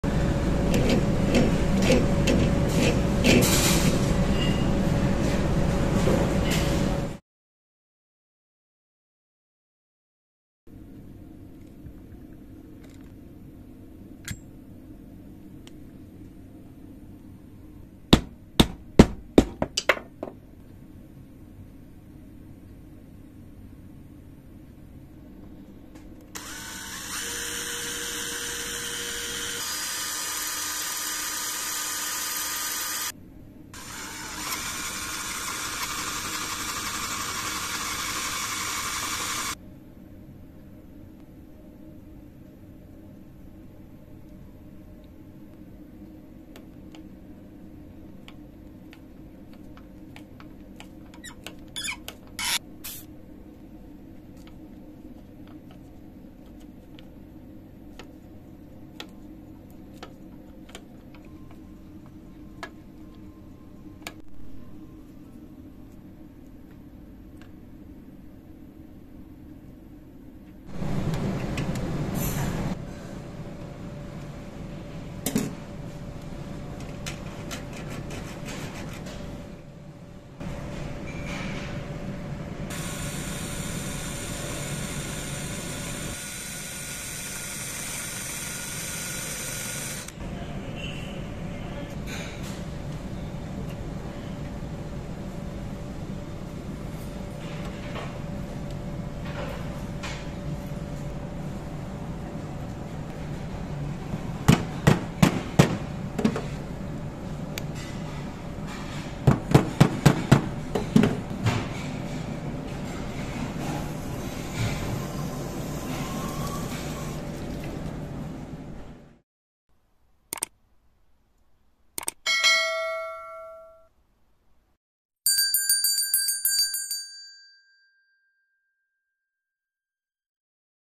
How To Fix a Spinning Tap Knob
Hi everyone! In this video, I'll show you how to fix a spinning tap knob.
It is a commercial faucet, and it is very difficult to buy a replacement cartridge and knob. Replacing the entire faucet will be very costly. How to fix it with almost one hand? The movie is a bit shaky, just because I filmed the process with my left hand :)